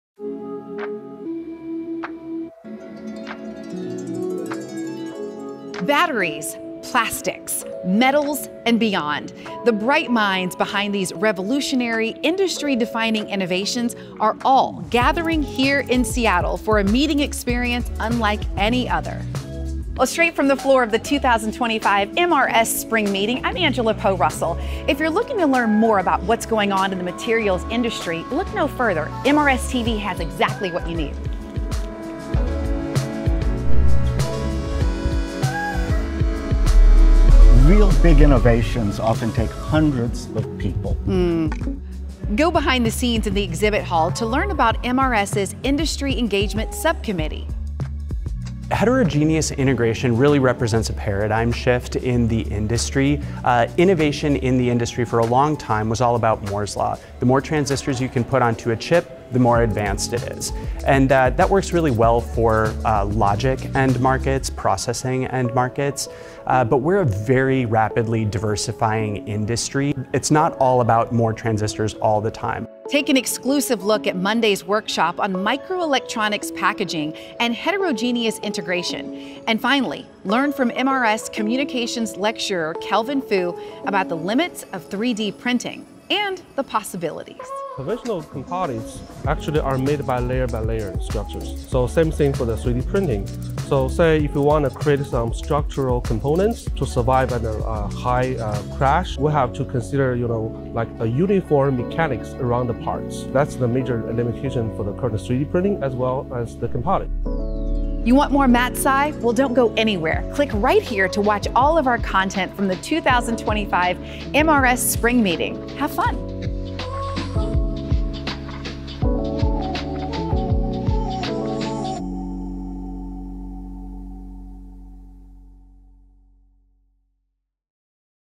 Dive Into the Materials Industry on MRS TV
Industry partnerships are an invaluable piece of the Materials Research Society's work. See how MRS TV is highlighting industry's role in the MRS 2025 Spring Annual meeting through exciting interviews and exclusive coverage:
Rethinking 3D Printing with Kelvin Fu
Heterogeneous Integration's Strengths and Weaknesses with Erik Hadland
Behind the Scenes at MRS Spring 2025's Exhibit Hall